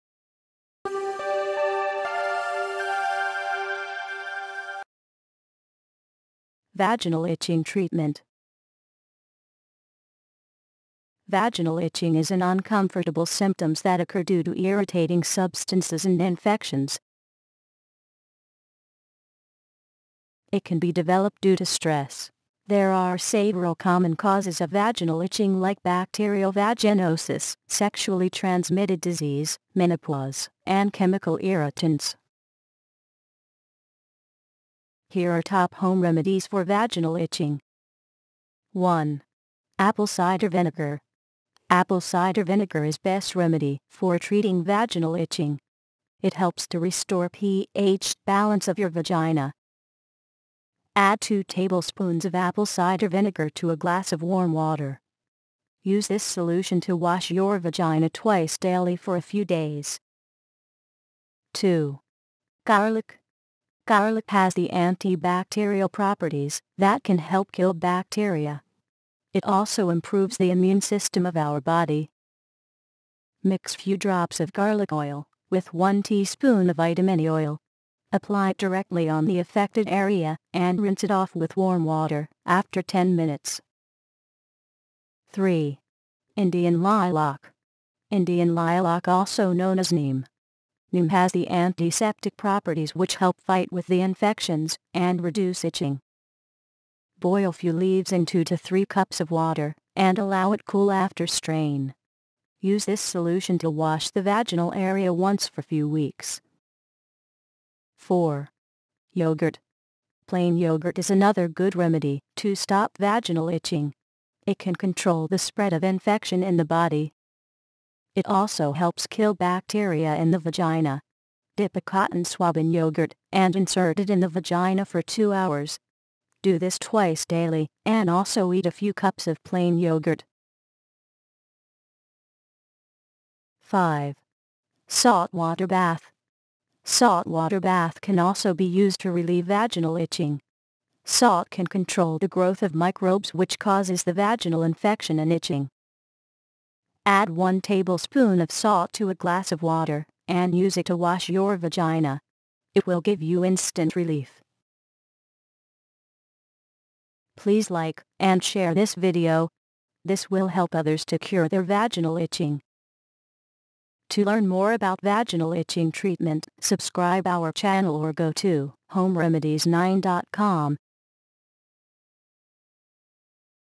How To Prevent Vaginal Itching - Best Treatment For Vaginal Itching
Yeast Infection --

Vaginal itching is an uncomfortable and more painful symptom can be occur due to infections and irritating substances. Common chemical irritants like soap, feminine sprays and creams can be causing of vaginal itching. There can be redness and swelling in and around the vagina in addition to itching and burning sensations. Woman also have discomfort during urination and intercourse. In this video we are going to discuss about how to prevent vaginal itching and best treatment for vaginal itching. Take care.

What causes vaginal itching?
Vaginal itching relief
How to stop vaginal itching
How to treat vaginal itching

Home Remedies For Vaginal Itching:-
1. Honey:-
Unpasteurized honey is best remedy to cure itching. Honey has antibacterial and antifungal properties that help to get rid of any microbial infection that could be causing the problem. Directly apply honey on the affected area and leave it for few minutes. Then take a bath in warm water. Repeat at least twice a day.
2. Garlic:-
Garlic contains antibacterial and antibiotic properties that can help kill bacteria. It helps the body to fight with infections. Mix few drops of garlic oil with vitamin E oil and apply this solution to the affected area. Wait for few minutes and then rinse with warm water. Use this remedy twice daily.
3. Tea Tree Oil:-
Tea tree oil has strong antibacterial properties. This essential oil is very effective in treating yeast infection. Directly apply tea tree oil on the affected area and leave it for some time. Then wash it off with warm water.
4. Boric Acid:-
Boric acid is very effective anti-fungal agent. Boric acid can be danger for your body. So use it very carefully at home. Mix ¼ tablespoon boric acid with one cup of water. Use this solution to rinse your vagina.
5. Coconut oil:-
Coconut oil has antiviral and antimicrobial properties. This oil can make skin soft from dryness. Mix 3-4 tablespoon of coconut oil to 6-8 cups of water. Rinse your vagina with this mixture. Consume 2-3 tablespoon of coconut oil daily.